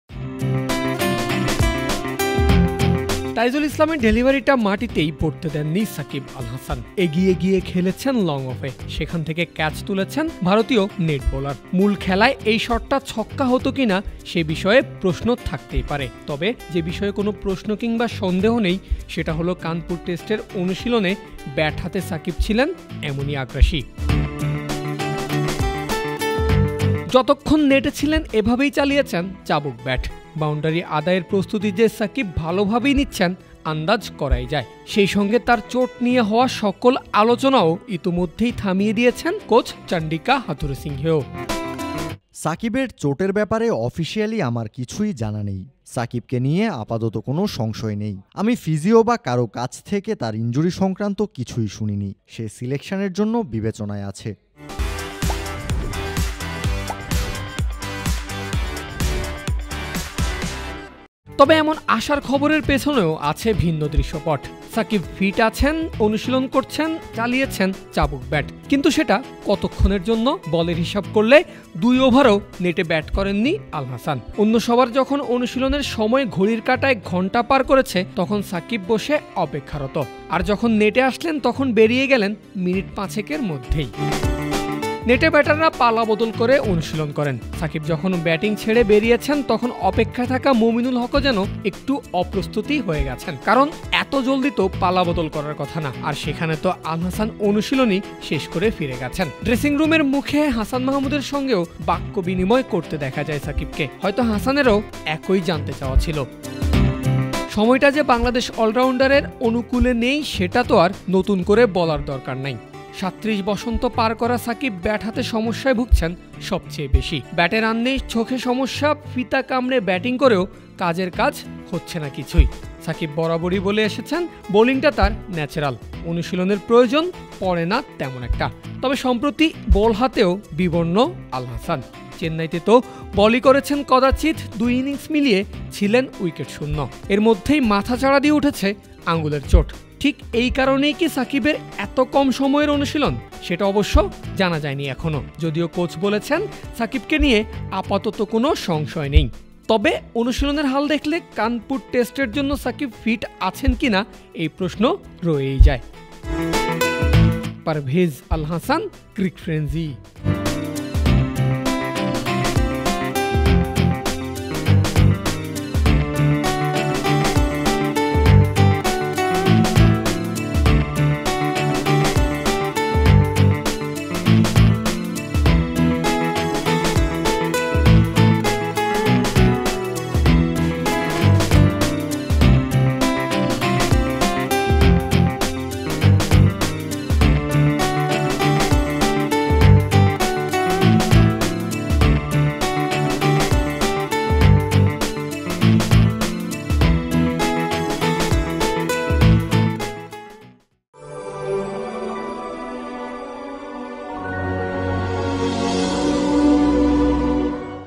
নেটে সাকিবের ধুমধাড়াক্কা ব্যাটিং, তবে অনুশীলন করেছেন কেবল ৫ মিনিট! সাকিবকে নিয়ে সংশয় নেই হাথুরুর!
নেটে সাকিবের ধুমধাড়াক্কা ব্যাটিং, তবে অনুশীলন করেছেন কেবল ৫ মিনিট! সাকিবকে নিয়ে সংশয় নেই হাথুরুর, তবুও থেকে যাচ্ছে প্রশ্ন